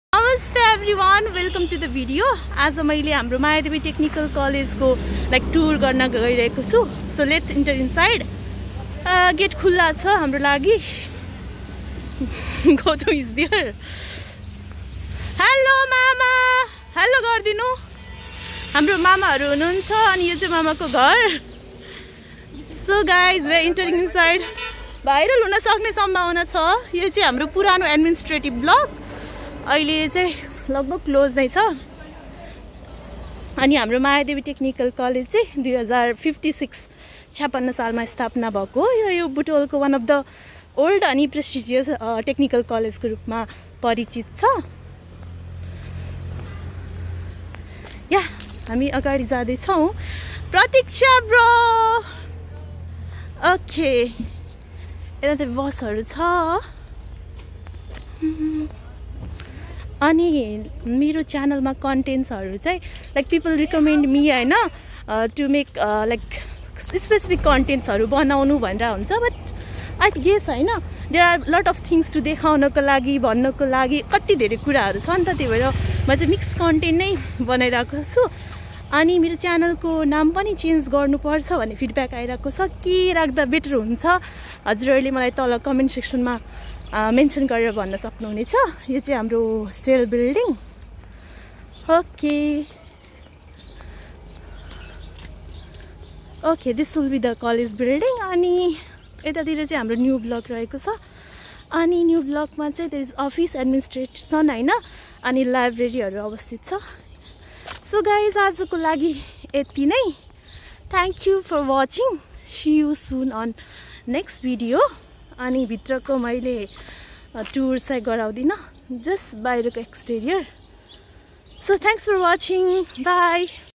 MAYADEVI TECHNICAL COLLEGE ||📚 BUTWAL |COLLEGE LIFE || Best colleges after SEE ||Nepali Students🏫❤️
This video is all about my college Mayadevi Technical College which is situated at Butwal , Dhawaha . It contains outdoor tour of the college surrounding . Institute in which we study always remains close to the heart . It's created so that we could view it after years .